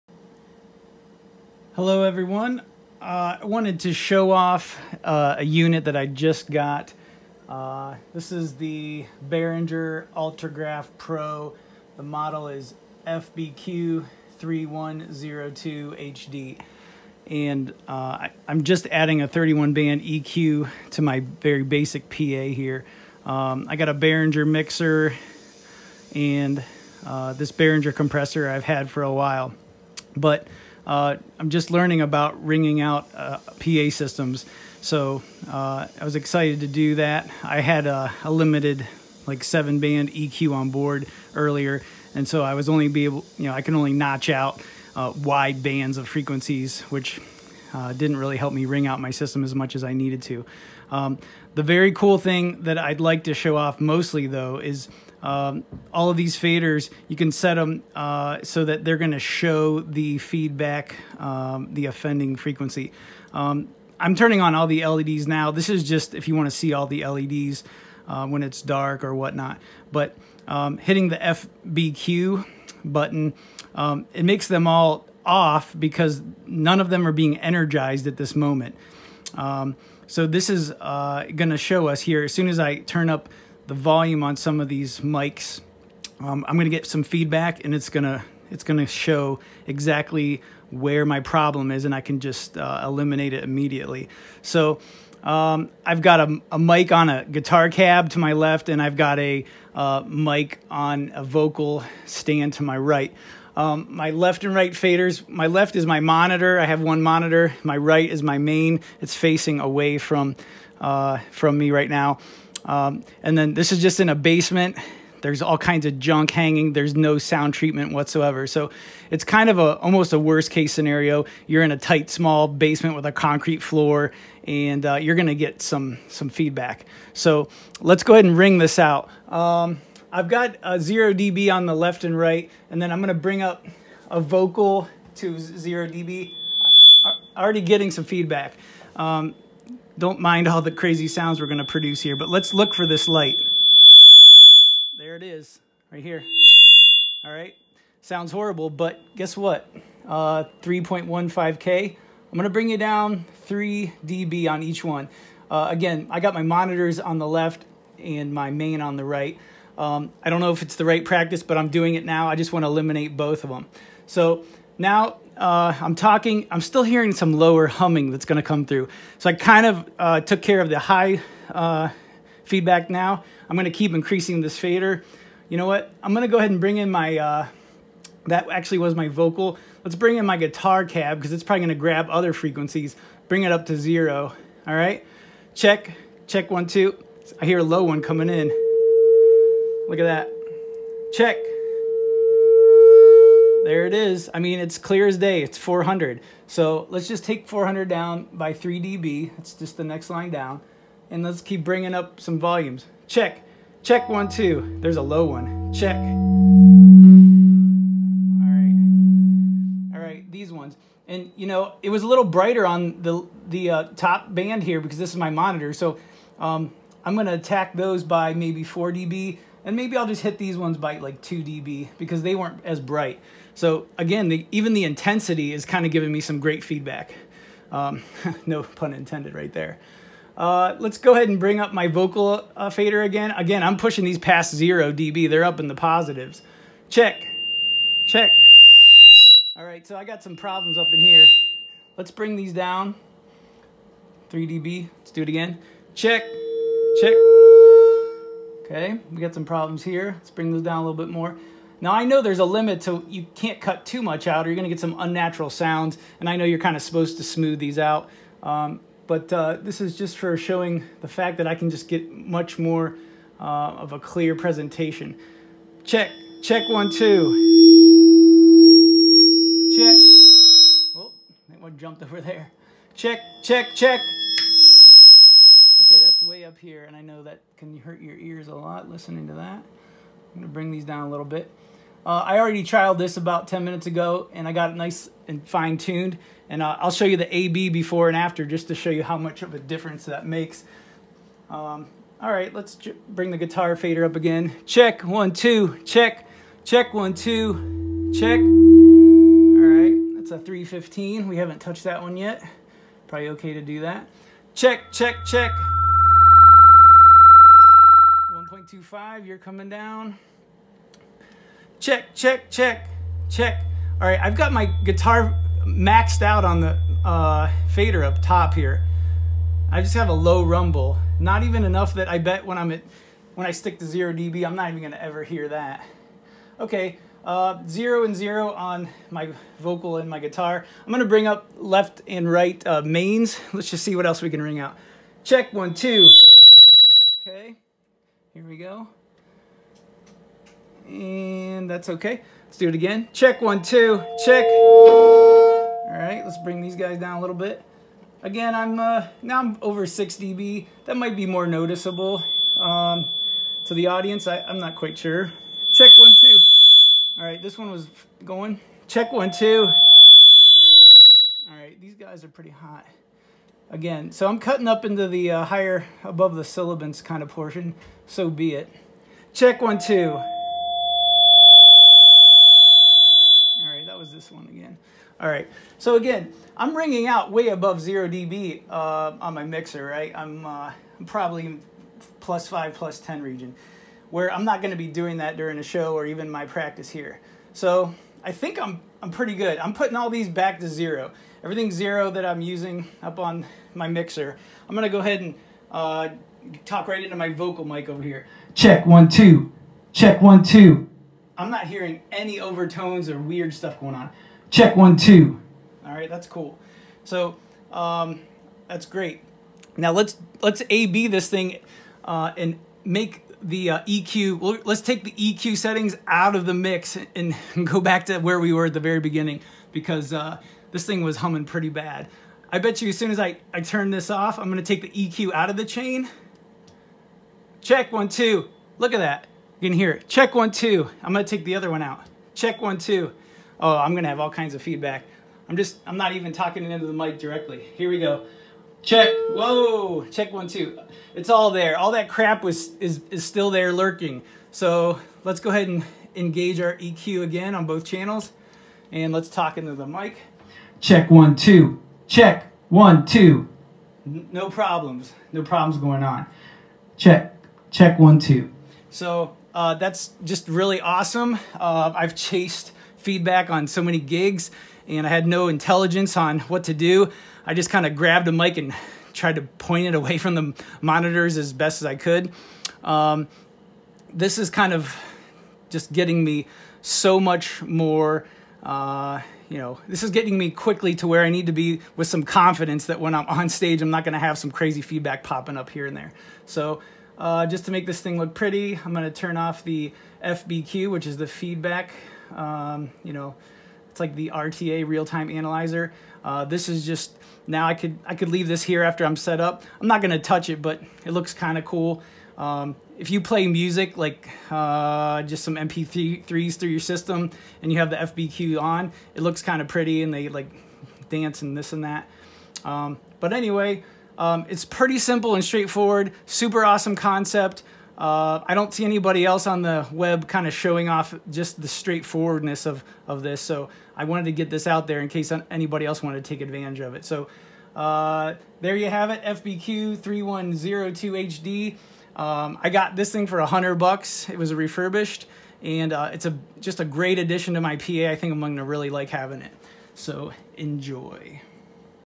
Ring out PA system with Behringer FBQ3102HD 31 band equalizer feedback detection and elimination.
This unit allows me to quickly identify problem frequencies causing feedback in my PA system so that I can eliminate them right away.